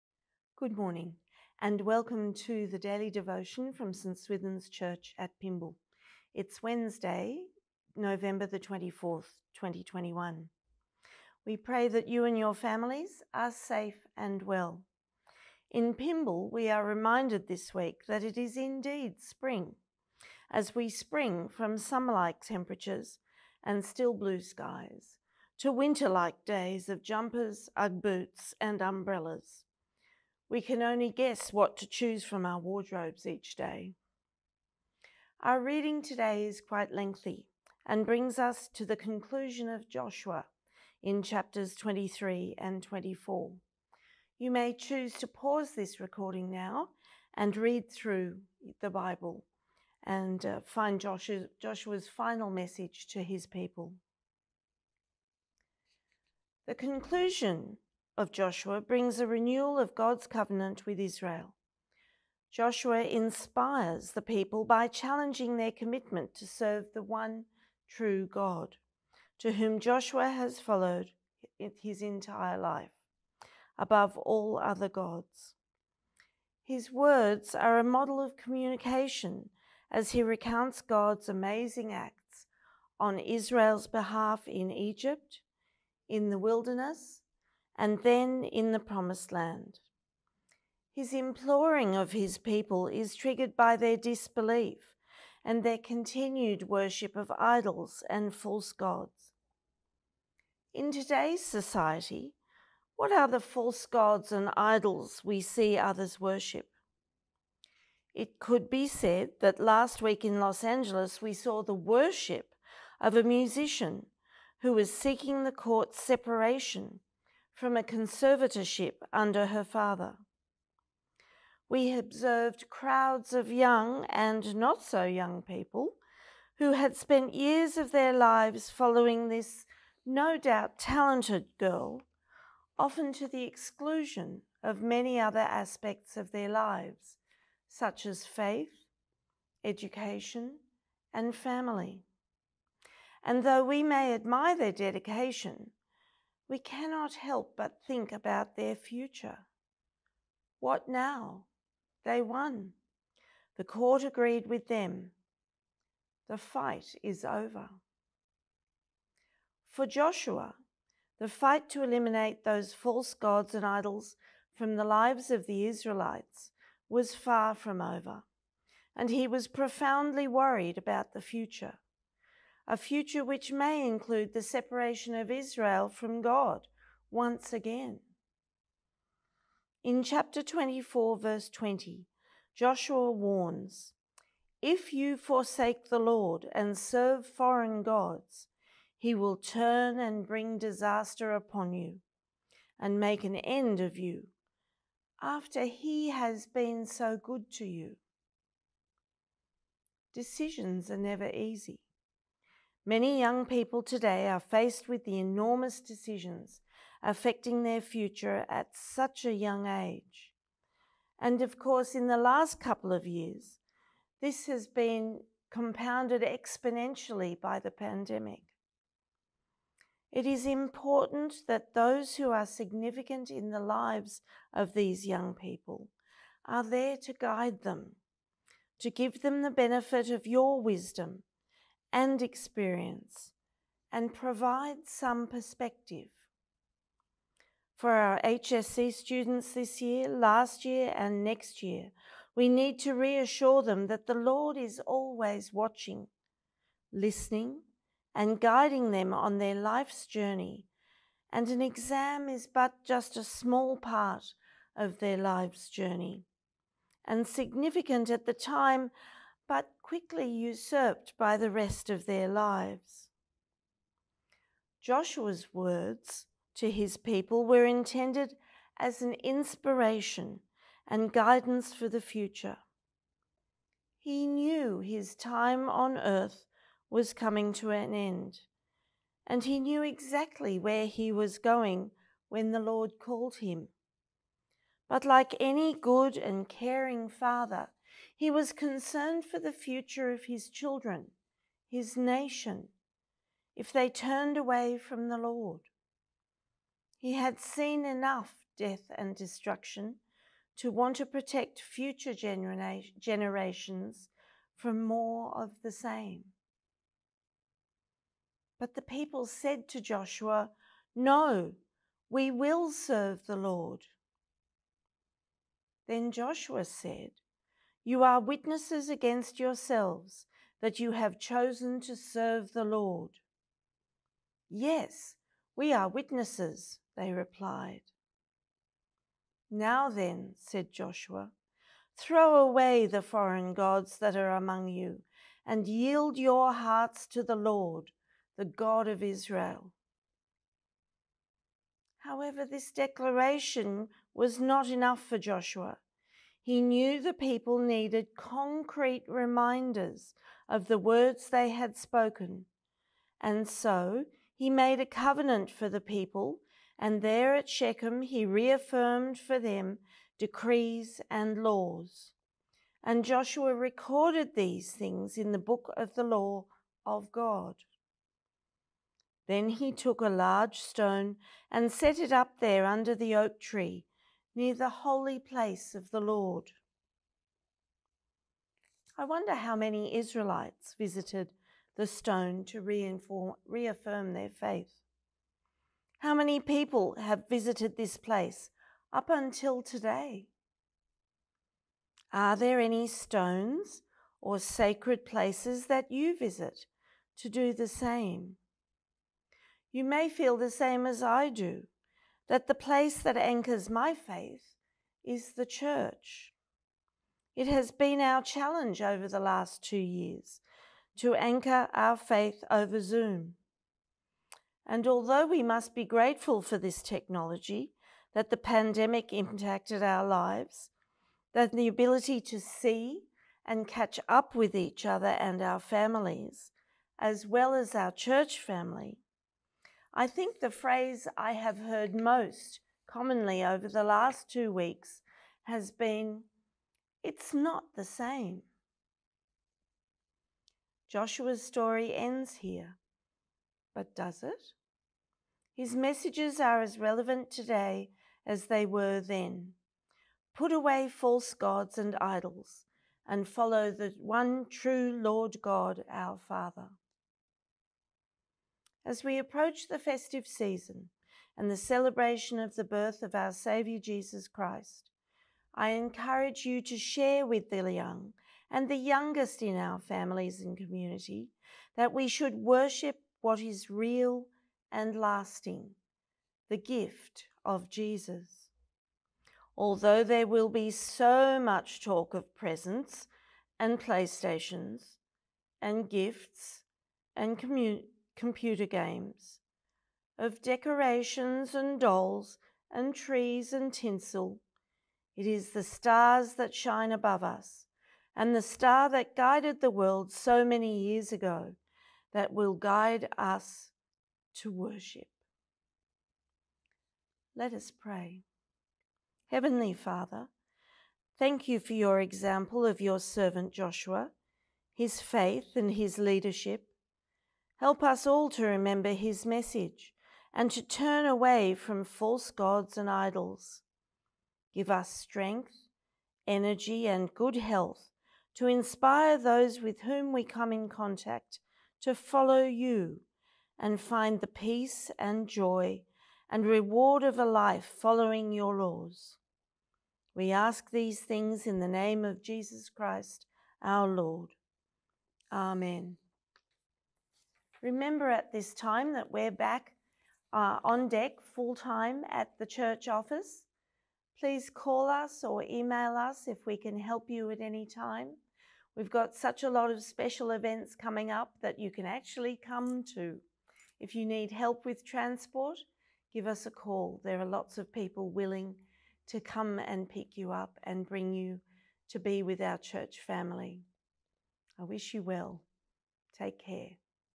Daily Devotion - Challenging False Idols
Join us this morning as Caroline looks into Joshua 23-24.